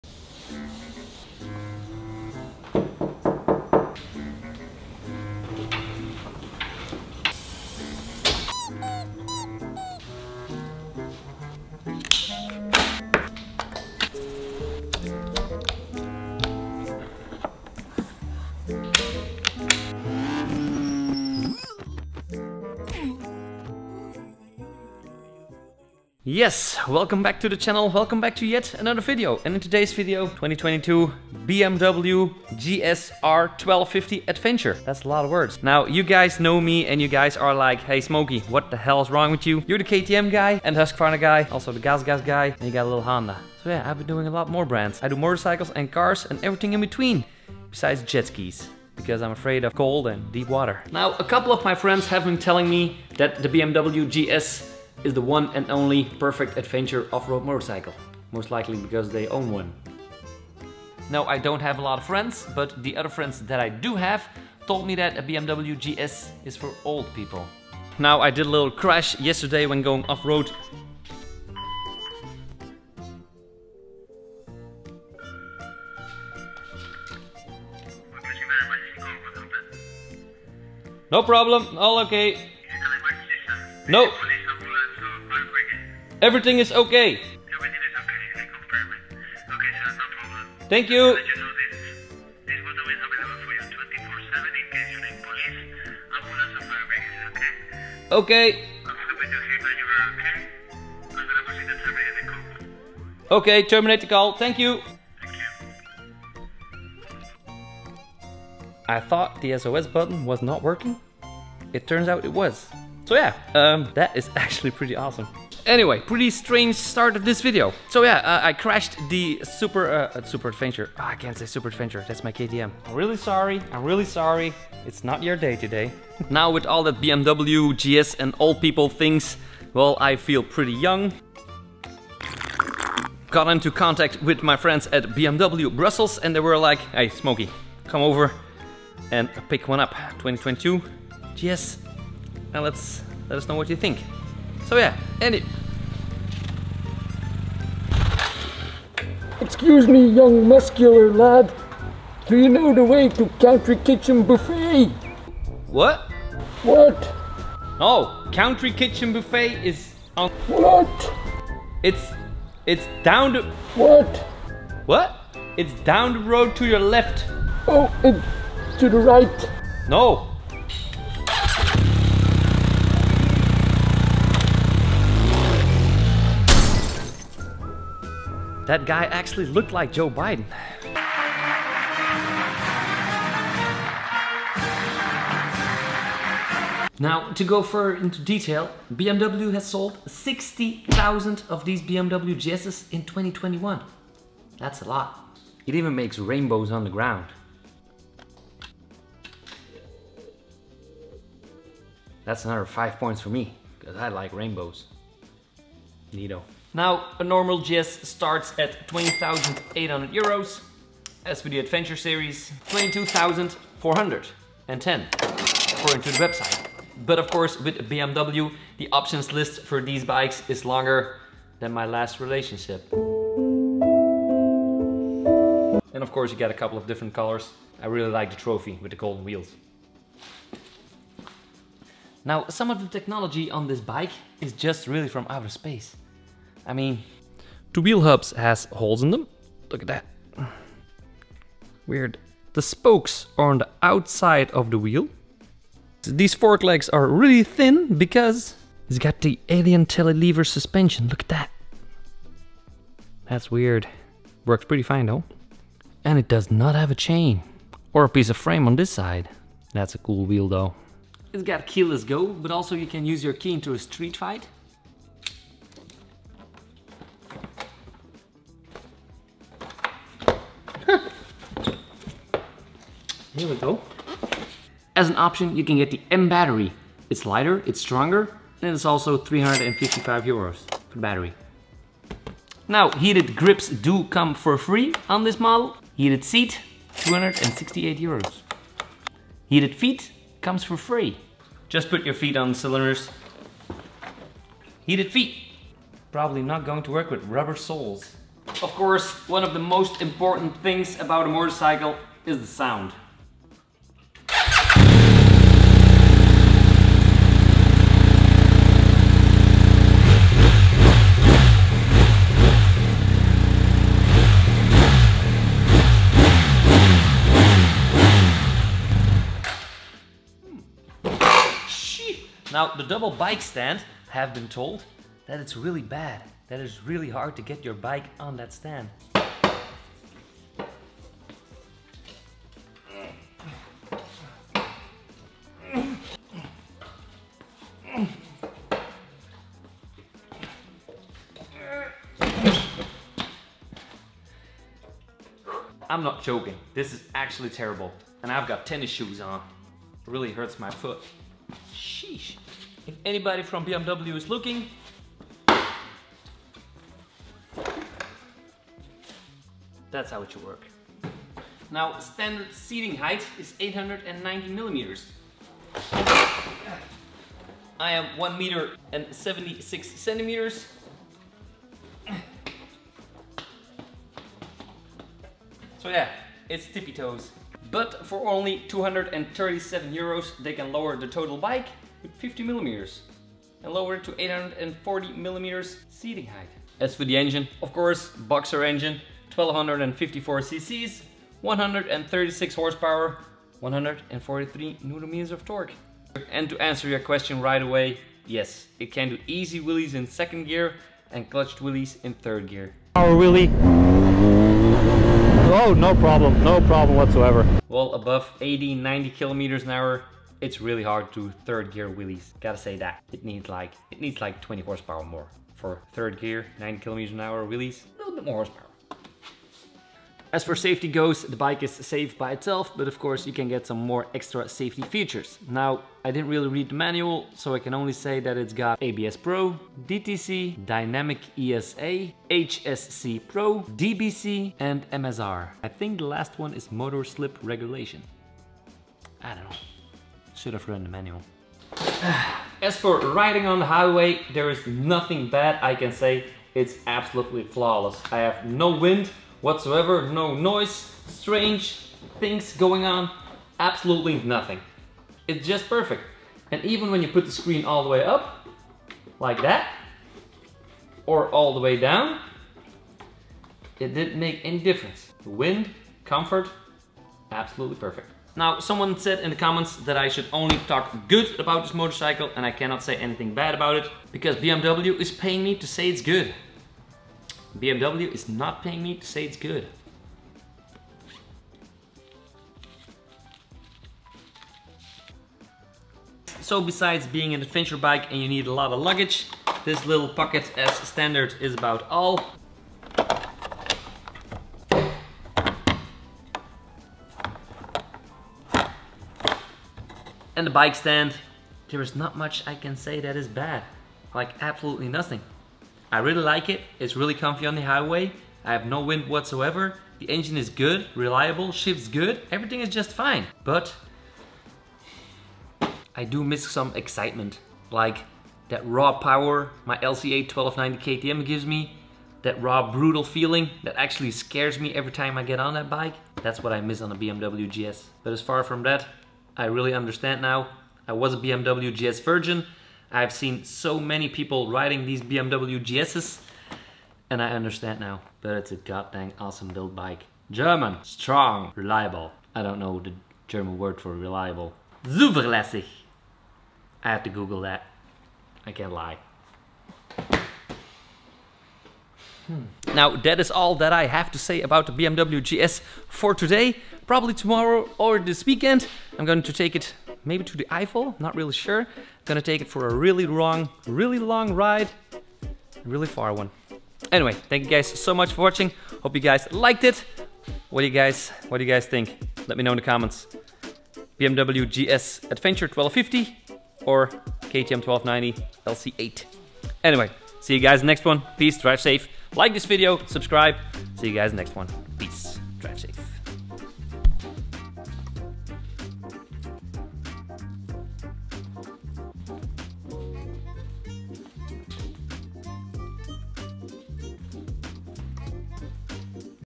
How Good is the NEW BMW R 1250GS Adventure? Full Review
In today's Life of Smokey review i'm taking a close look at the brand new 2022 BMW R1250 GS Adventure triple black from Bmw Benelux for the first time ever. It's a fun and comfy adventure motorcycle , nothing bad i can say about it besides the small storage in front and the terrible double stand.